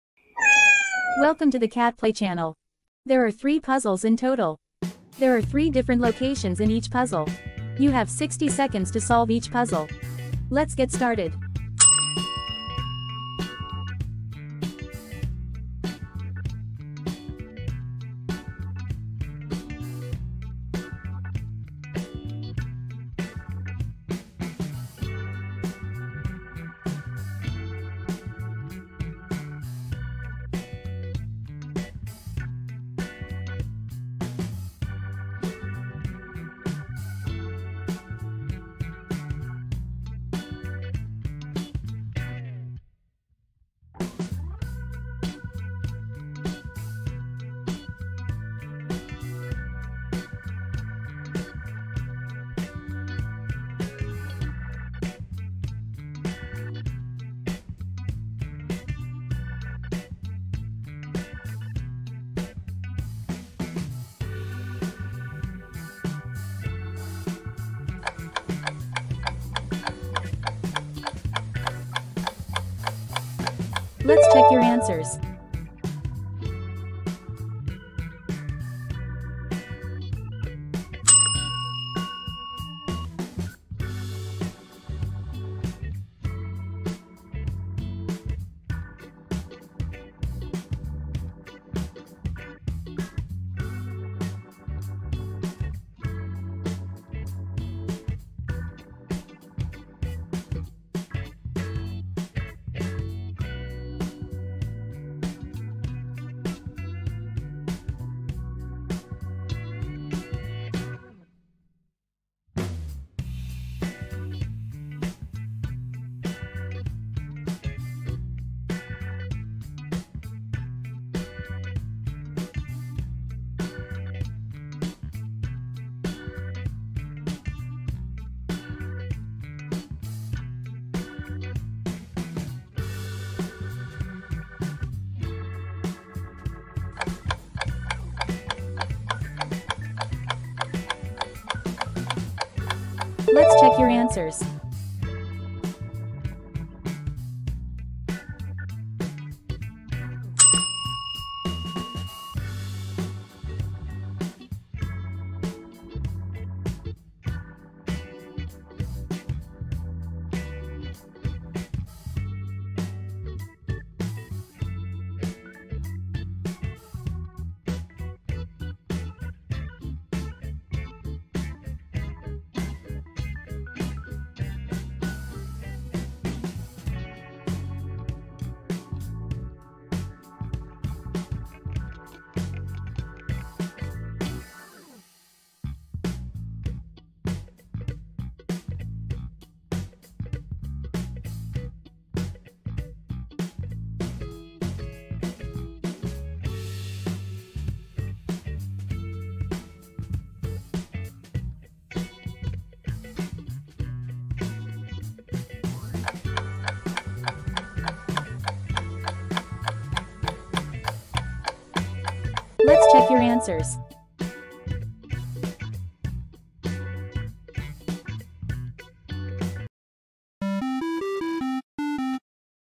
Spot The Difference 틀린그림찾기 /Only Genius Find Difference/ Find Painter & kitten
What are the lovely cats doing today? Where are the cute cats going? Their pictures make you wonder what they are doing.
To find out in the game, we took dozens of pictures of cute cats and created a very interesting image puzzle with our favorite cats!

There are 3 puzzles in the quiz and you need to find 3 different locations for each puzzle - can you solve all 3 puzzles featuring our lovely beautiful cats?

Have fun solving other photo-seeking puzzles featuring adorable cats. Have fun!